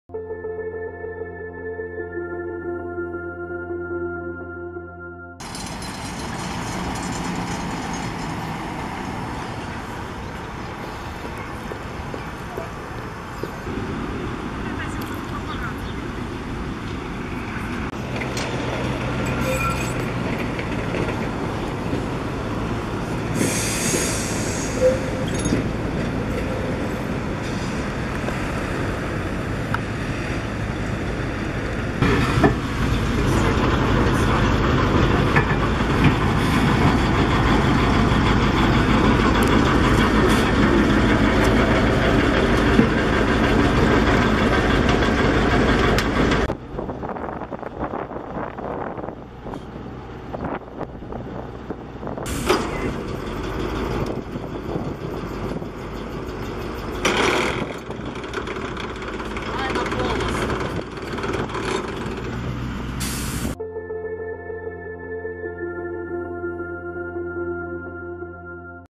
Утром в Мариуполе сошел с рельсов трамвай
Сегодня утром в Мариуполе на площади Кирова сошел с рельсов трамвай. В результате оказалось заблокировано движение трамваев.